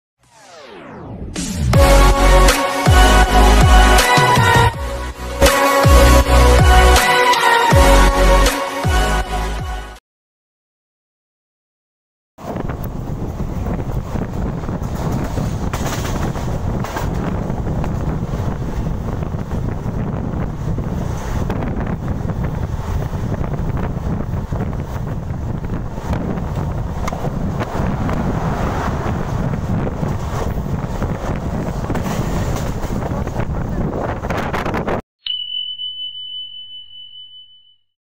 Emu overtakes bandra bhagat ki kothi express with brc wap 4
Bombardier local overtakes bandra bhagat ki Kothi express with brc wap 4e rake sharing with adhemdbad bhagat ki kothi express.